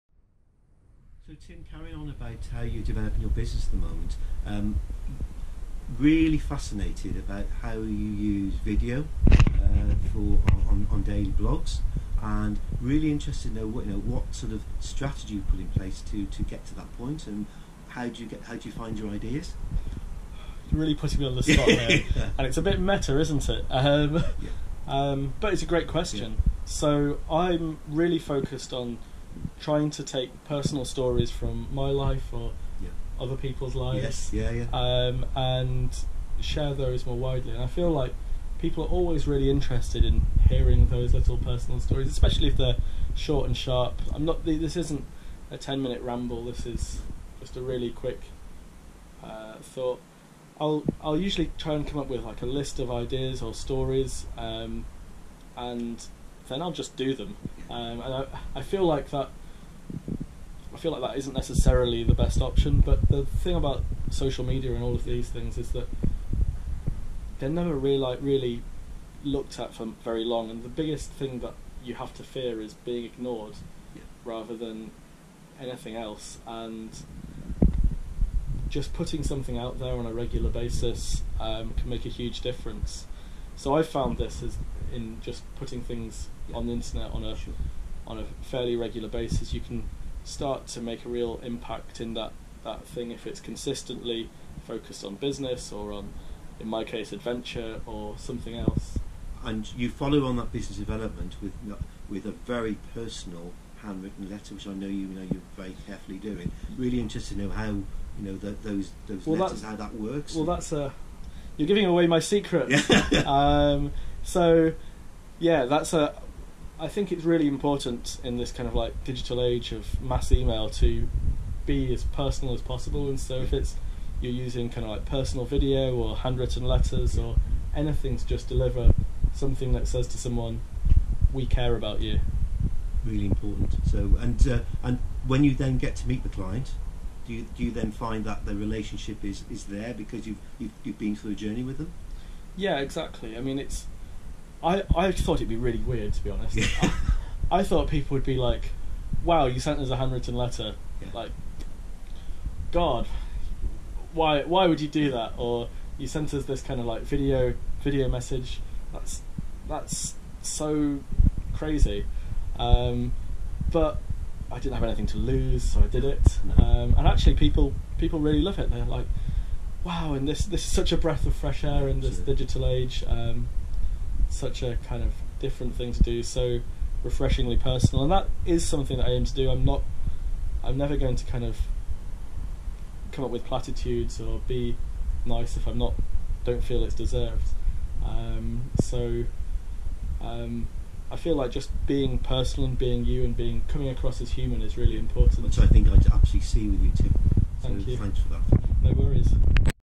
"How do you choose what to post to social media?"
AJ gets me to share my secrets